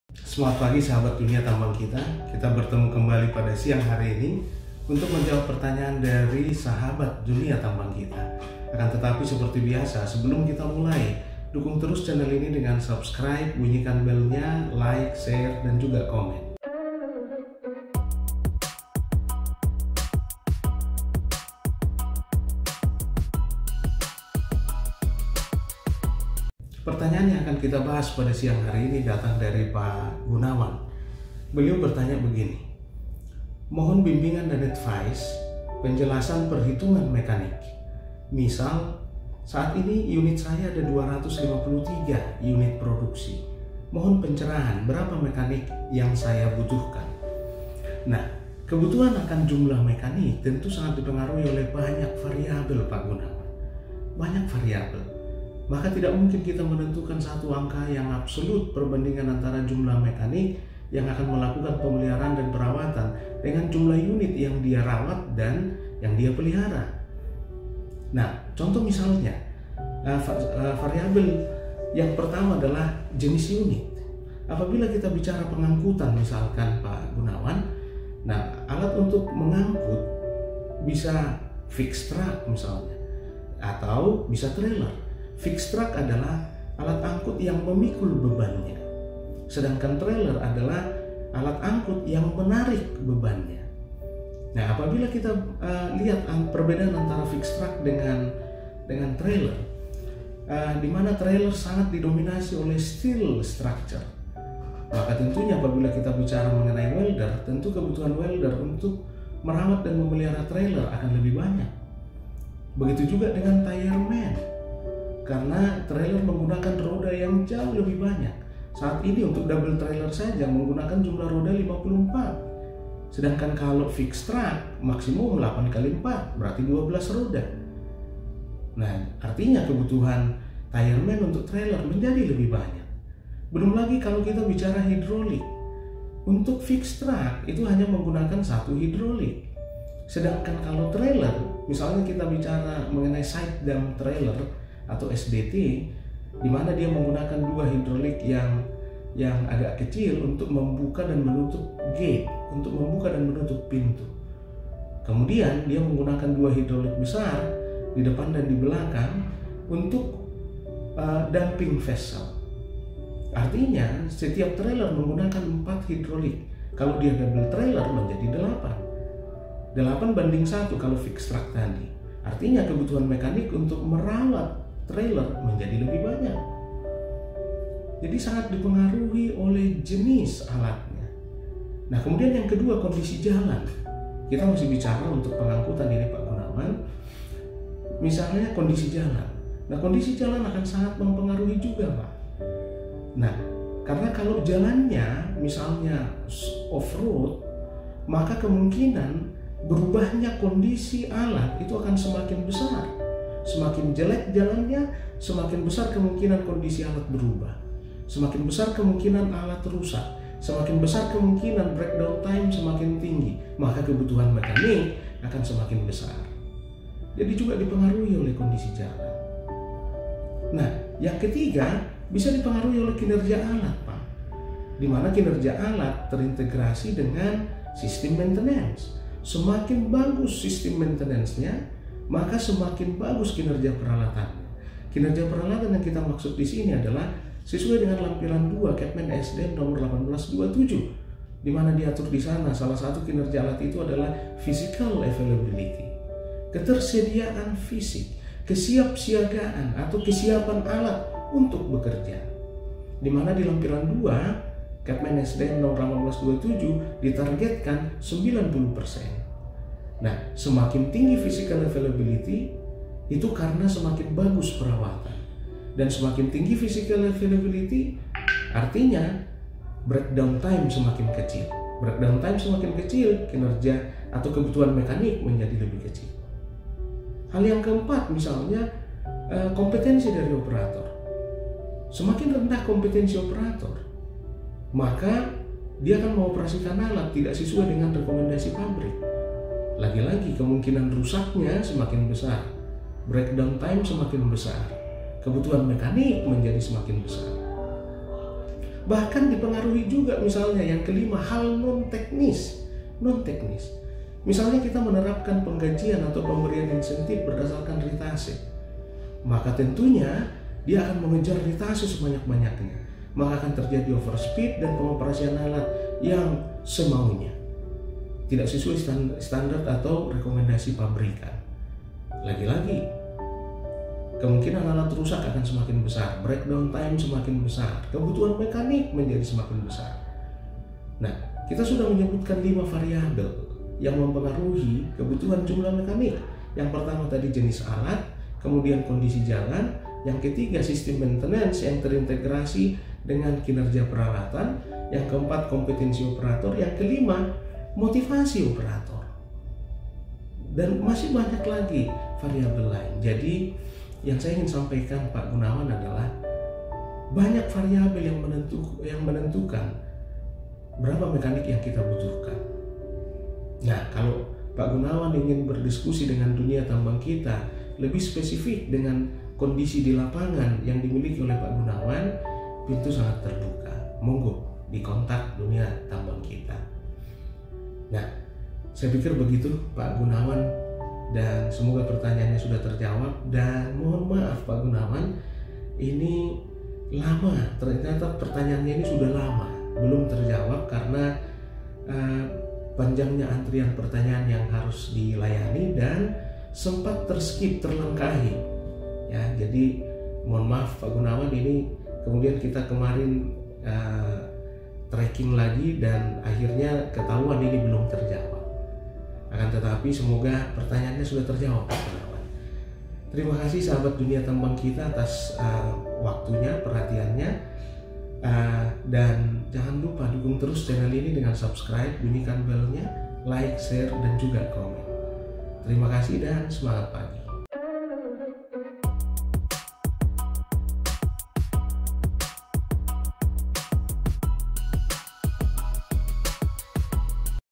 Perbandingan Jumlah Mekanik Vs Peralatan
Video ini dimaksudkan untuk menjawab pertanyaan sahabat "Dunia Tambang Kita" khususnya terkait Perbandingan antara "Jumlah Mekanik Vs Peralatan" yang ideal.

Semoga bermanfaat.